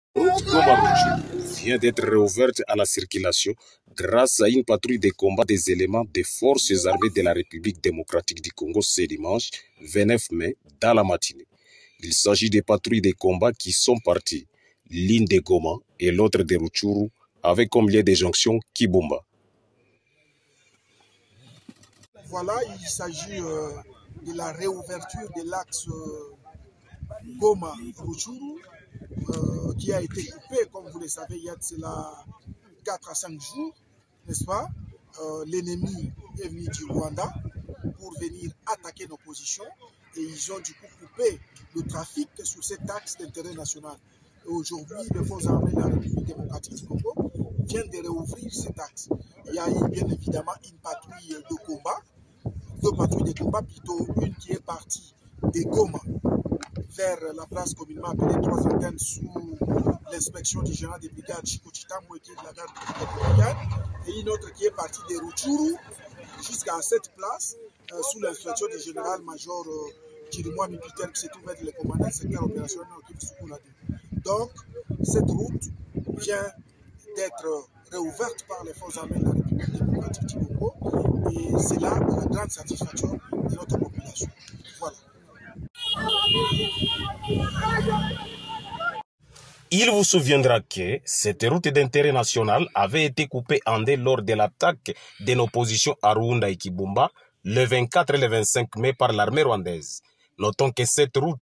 FARDC VICTOIRE SUR L'AGRESSION DE KAGAME SUR LA ROUTE GOMA RUTSHURU VOUS VOYEZ ICI LES FDLS ?!!!
FARDC VICTOIRE SUR L'AGRESSION DE KAGAME SUR LA ROUTE GOMA RUTSHURU VOUS VOYEZ ICI LES FDLS ?!!!  nos force armée de termine et soutenus par son peuple continue à barre la route à la force de Kagame et son esprit de va t'en guerre